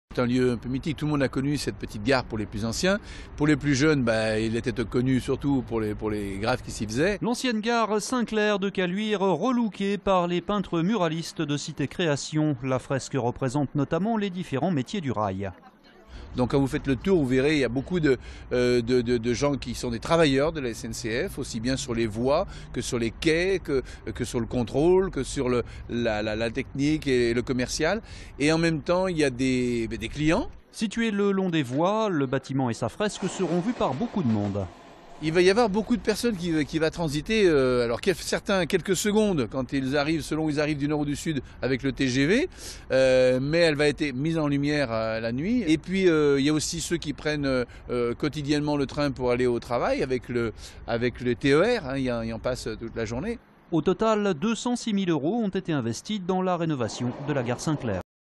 La gare Saint Clair relookée !
Sites webs/blogs, copiez cette vidéo avec le bouton "Intégrer" !
 L'ancienne gare Saint Clair de Caluire a été relookée par les peintres muralistes de Cité Création.